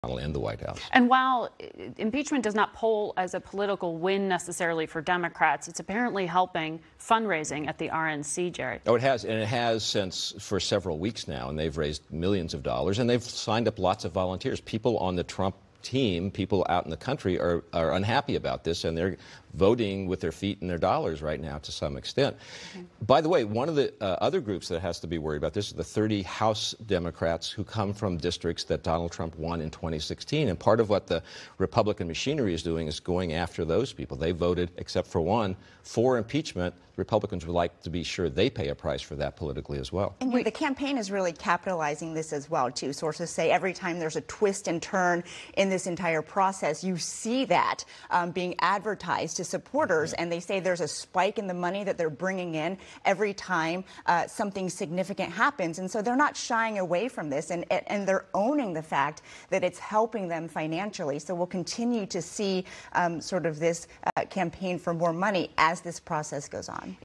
WSJ’s Seib: People Are “Voting With Their Feet And Their Dollars” Against Impeachment
The Wall Street Journal’s Gerald Seib notes how people are “voting with their feet and their dollars” against impeachment, leading to record fundraising for Republicans for 2020.